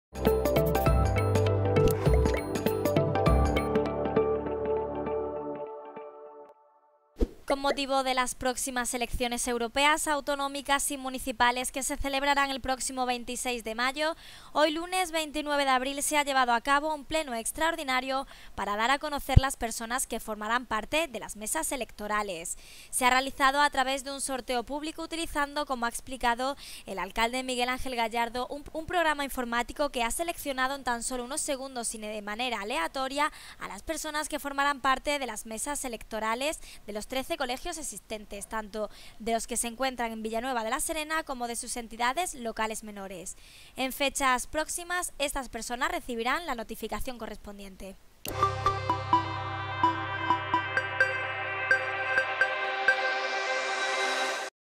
Elegidas a través de soreto las personas que formarán parte de las mesas electorales del 26 de mayo
Se eligen, a través de sorteo, las personas que formarán parte de las mesas electorales del 26 de mayo
Se ha realizado en un pleno extraordinario celebrado hoy lunes 29 de abril